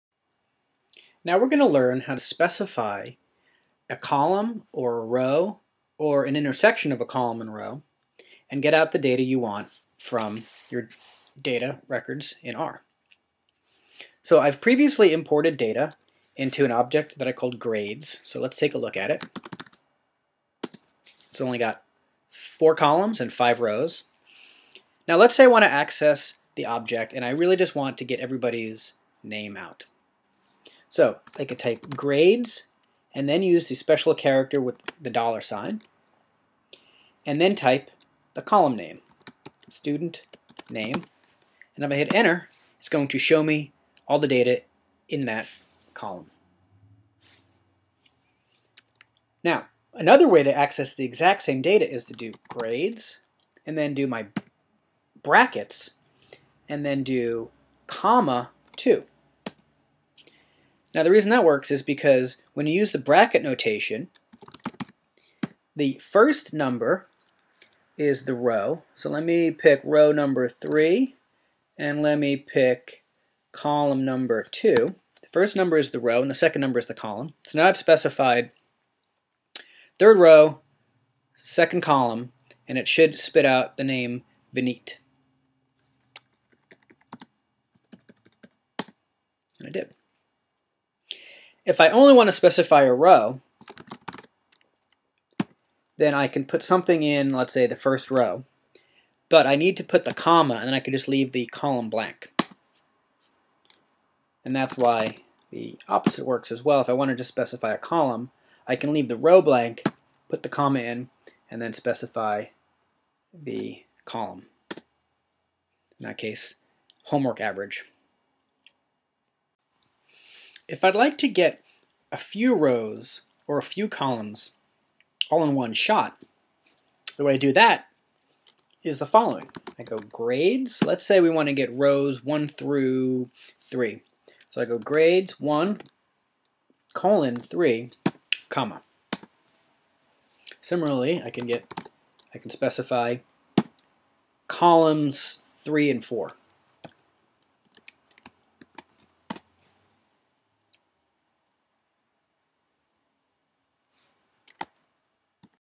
R Statistical: Accessing Columns and Rows
Learn how to select a column, a row, or an individual data points in R.